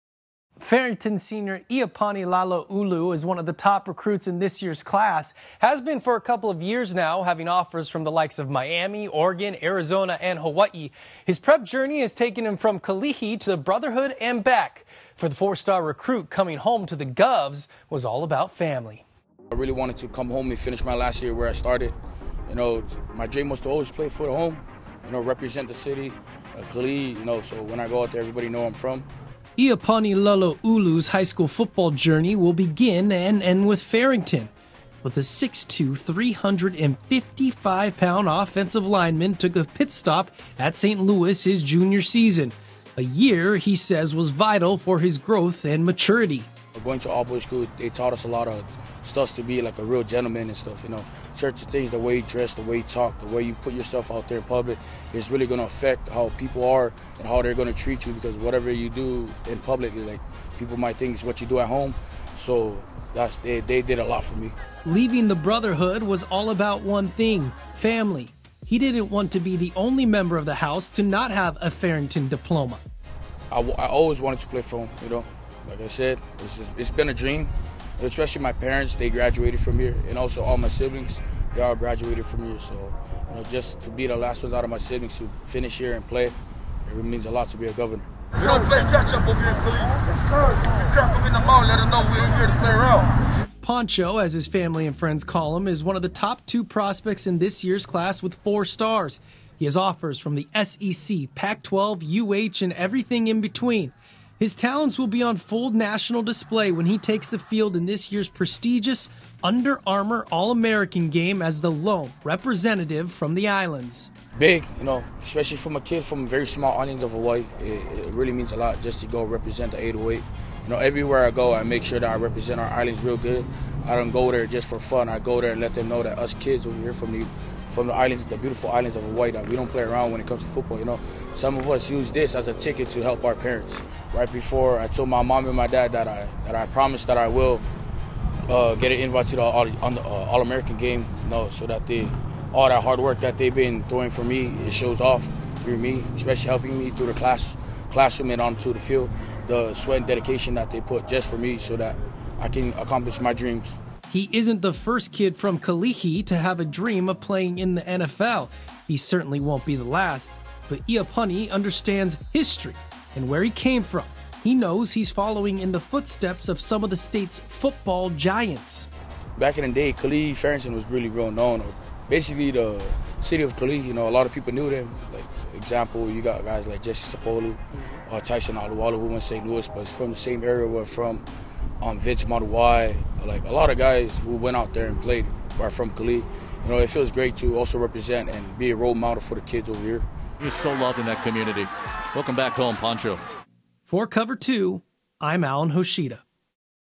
FOR LIFE: Iapani Laloulu return to Farrington is all about family
Iapani Laloulu's high school football journey will begin and end at Farrington High School, but the 6'2, 355 lbs. offensive lineman took a pit stop at Saint Louis for his junior season. He says it was a vital year for his growth and maturity.